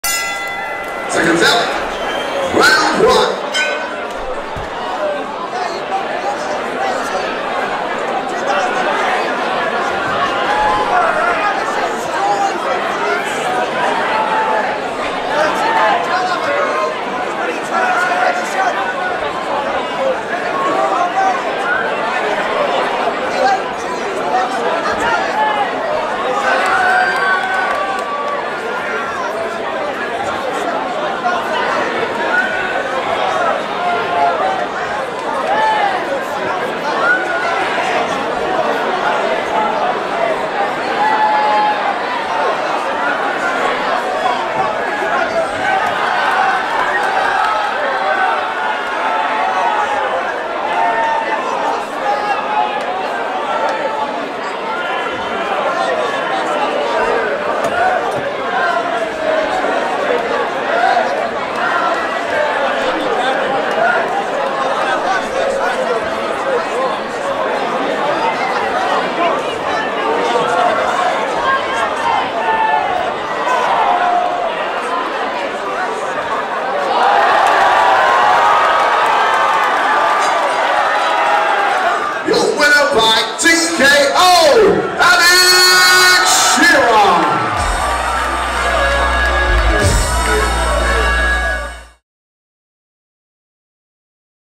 Fight 9 Alex Shearer red corner vs Cody Helme blue Corner
BOXCLEVER BOXING - April 22nd 2022